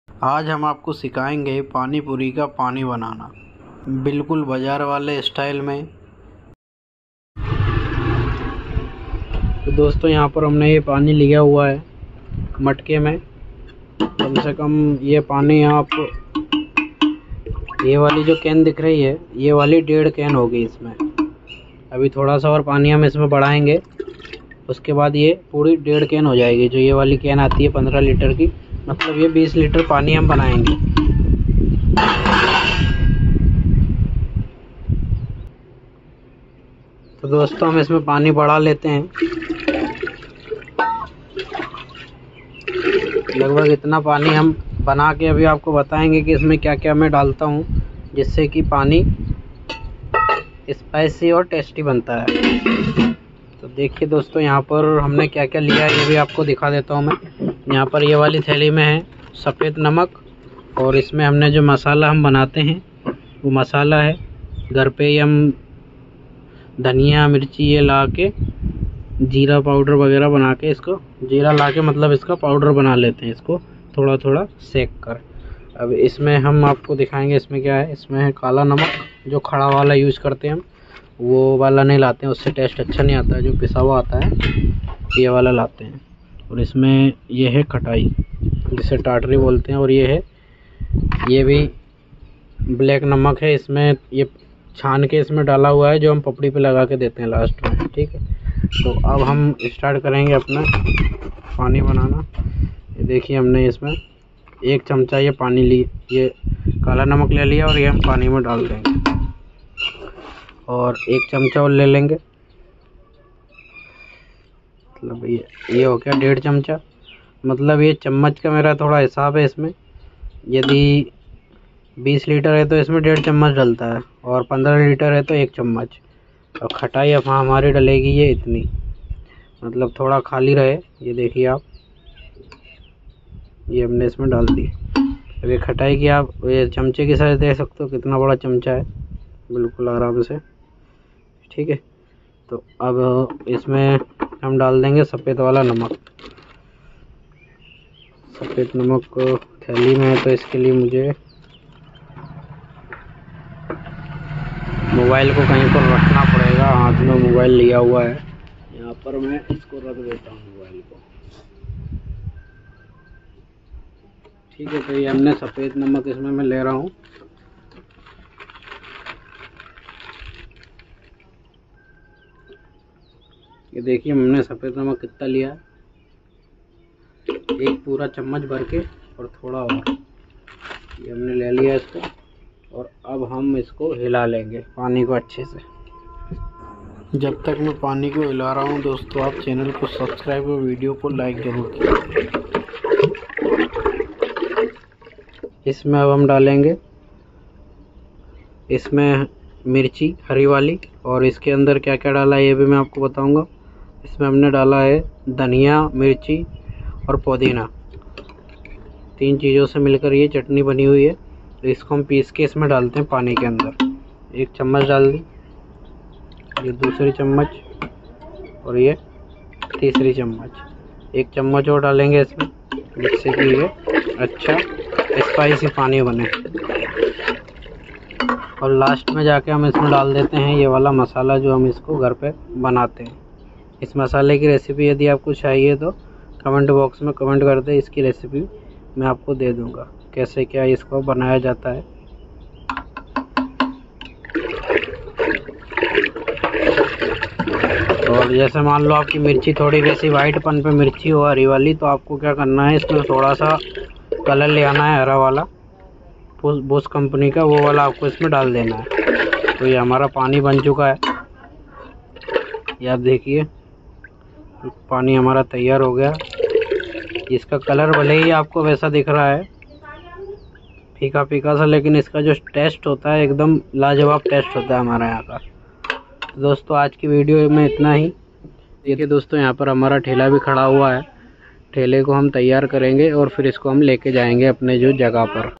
पानी पुरी का पानी कैसे बनाएं || Pani Puri ka Pani kaise banaye
घर पर बने बाजार जैसा पानी पुरी का पानी
👇👇👇 घर पर पानी पुरी का पानी बनाने के लिए यह वीडियो देखें